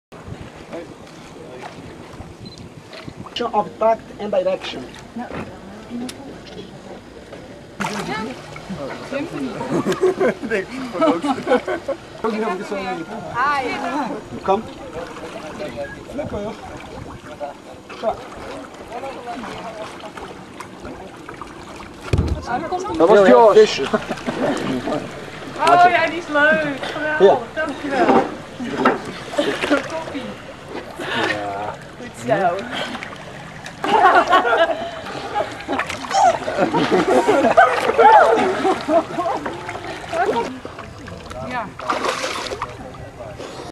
Swimming with boto dolfins 2008
This video was upscaled with Topaz AI

Nabij Manaus in Brazilië konden we visjes voeren aan boto dolfijnen die daar in de Amazonerivier zwemmen.